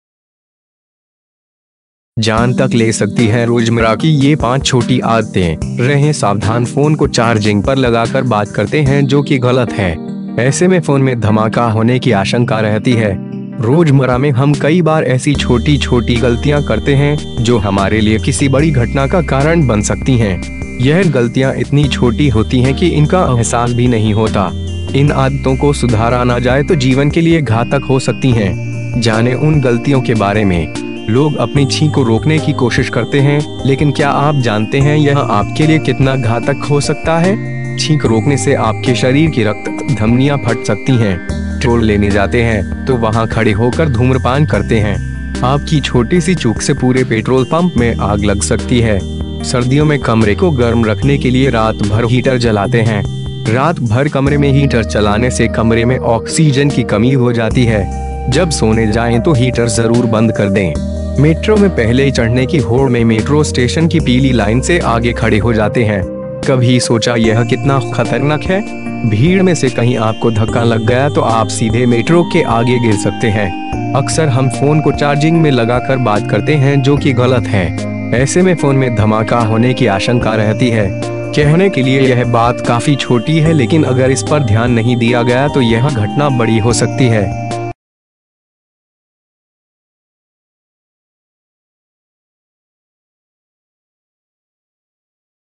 जान तक ले सकती हैं रोजमर्रा की ये 5 छोटी आदतें, रहें सावधान
फोन को चार्जिंग पर लगाकर बात करते हैं जो कि गलत है. ऐसे में फोन में धमाका होने की आशंका रहती है.

रोजमर्रा में हम कई बार ऐसी छोटी-छोटी गलतियां करते हैं, जो हमारे लिए किसी बड़ी घटना का कारण बन सकती हैं. यह गलतियां इतनी छोटी होती हैं कि इनका अहसास भी नहीं होता. इन आदतों को सुधारा ना जाए तो जीवन के लिए घातक हो सकती हैं. जानें उन गलतियों के बारे में.

लोग अपनी छींक को रोकने की कोशिश करते हैं, लेकिन क्या आप जानते हैं यह आपके लिए कितना घातक हो सकता है? छींक रोकने से आपके शरीर की रक्त धमनियां फट सकती हैं.

ट्रोल लेने जाते हैं, तो वहां खड़े होकर धूम्रपान करते हैं. आपकी छोटी सी चूक से पूरे पेट्रोल पंप में आग लग सकती है

सर्दियों में कमरे को गर्म रखने के लिए रात भर हीटर जलाते हैं. रात भर कमरे में हीटर चलाने से कमरे में ऑक्सीजन की कमी हो जाती है. जब सोने जाएं तो हीटर  जरूर बंद कर दें.

मेट्रो में पहले चढ़ने की होड़ में मेट्रो स्टेशन की पीली लाइन से आगे खड़े हो जाते हैं. कभी सोचा यह कितना खतरनाक है? भीड़ में से कहीं आपको धक्का लग गया तो आप सीधे मेट्रो के आगे गिर सकते हैं.

अक्सर हम फोन को चार्जिंग में लगाकर बात करते हैं जो कि गलत है. ऐसे में फोन में धमाका होने की आशंका रहती है. कहने के लिए यह बात काफी छोटी है लेकिन अगर इस पर ध्यान नहीं दिया गया तो यह घटना बड़ी हो सकती है.